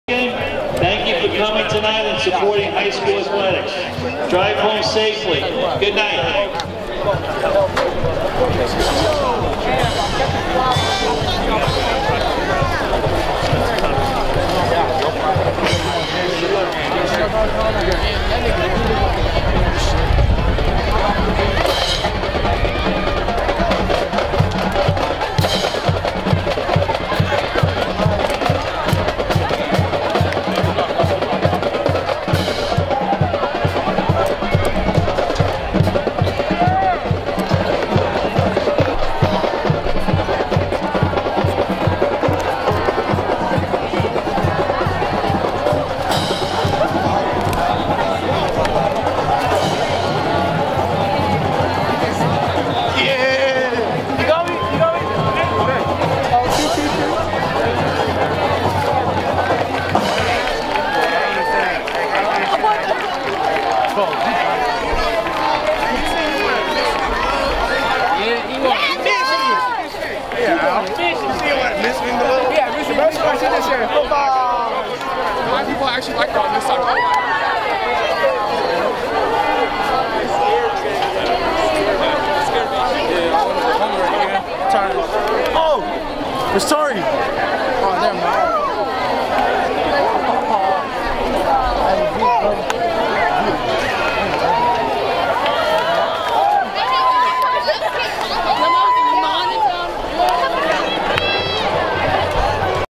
Post Game Handshake & Victory Walk | 8.30.19
Post Game Handshake & Victory Walk after St. John Bosco High School 56-21 over Don Bosco Prep on August 30, 2019 | Panish Family Stadium | Bellflower, CA